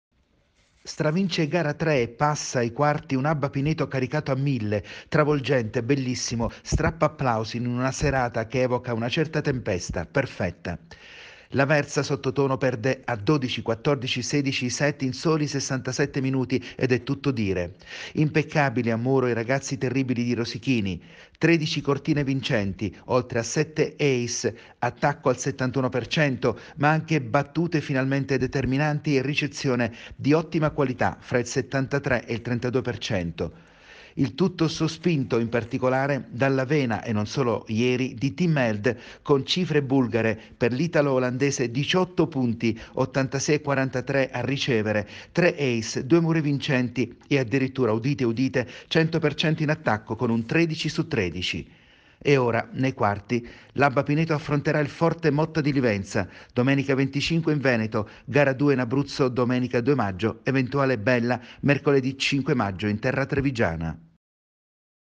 Volley. Pineto passa il turno. Ora c’è Motta di Livenza
Una Abba Pineto ai limiti della perfezione supera Aversa in tre set e passa il turno. Ora per i pinetese c’è l’arduo scoglio Motta di Livenza